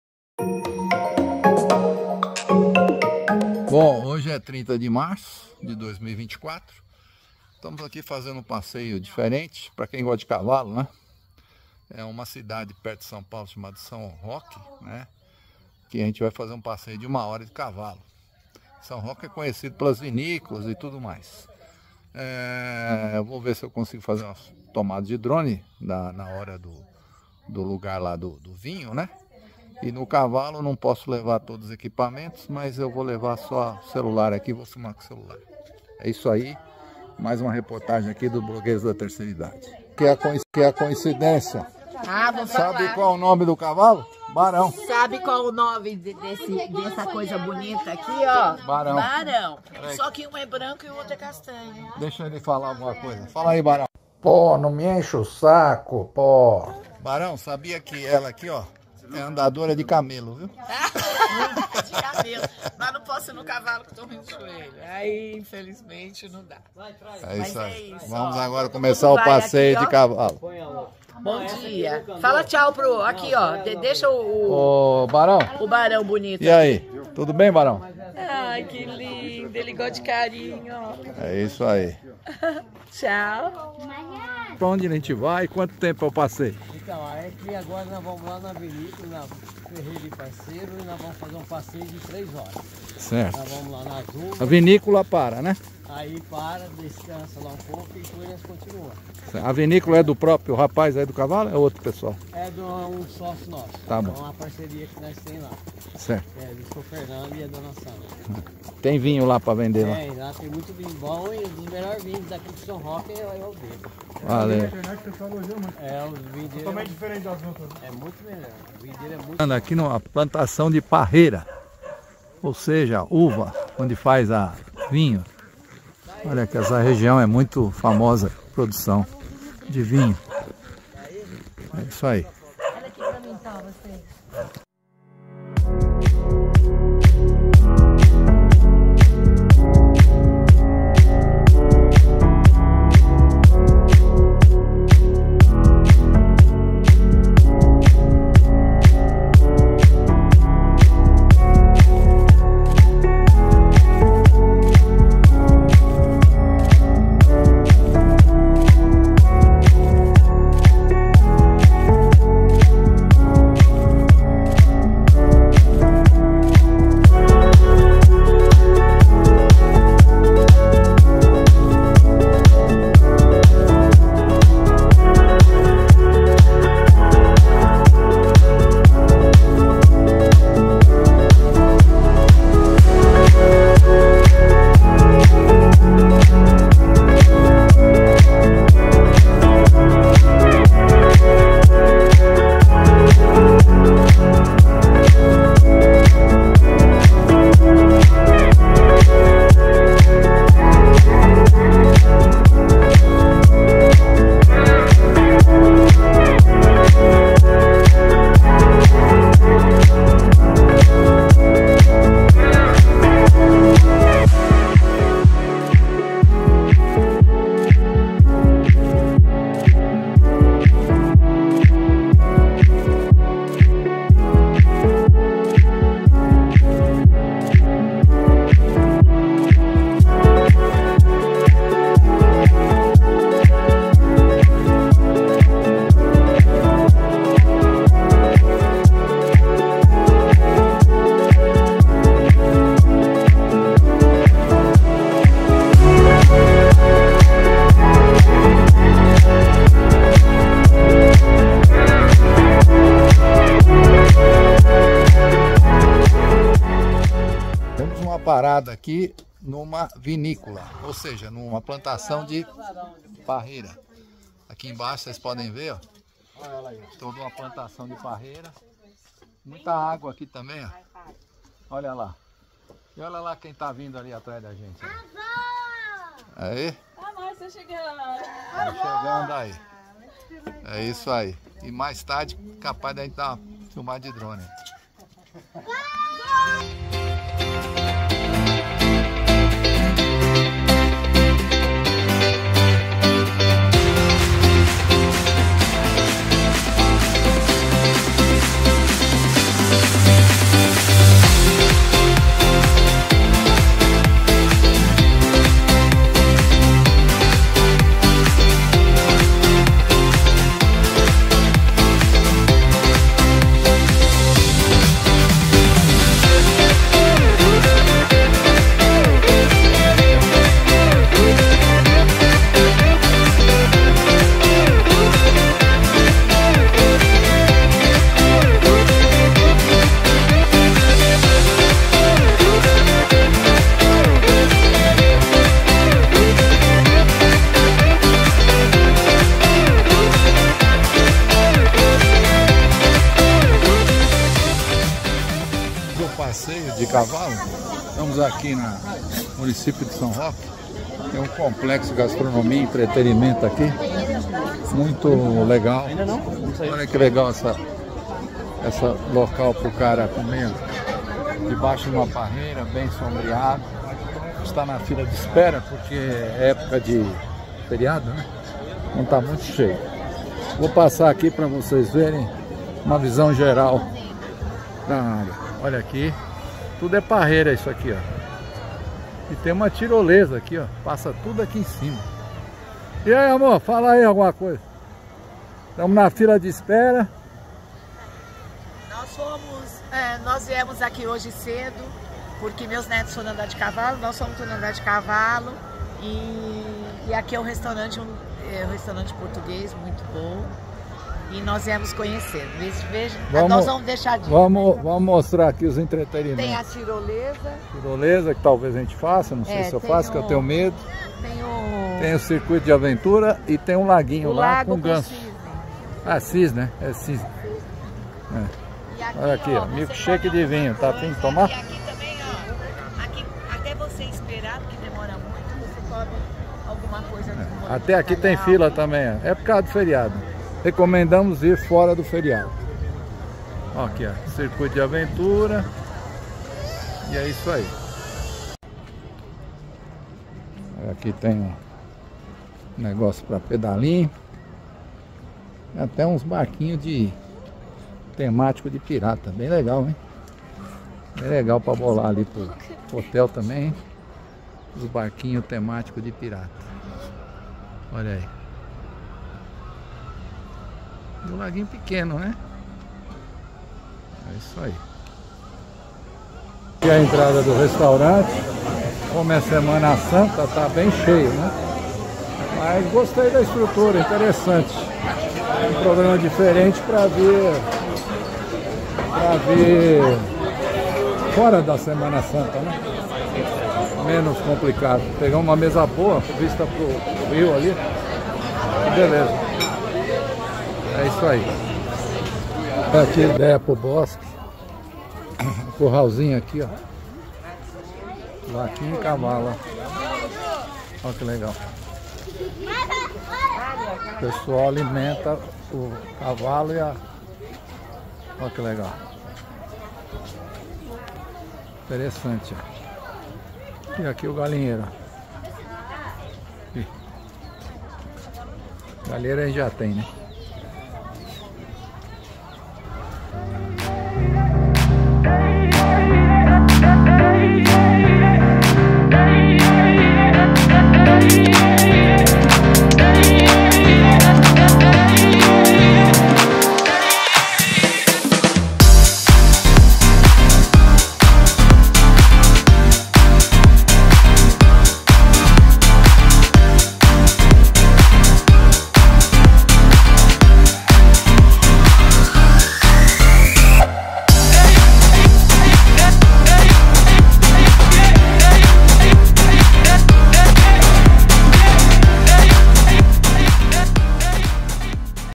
Passeio em São Roque - SP by Tv Tororomba
Cavalgada e restaurante temático. Marćo/24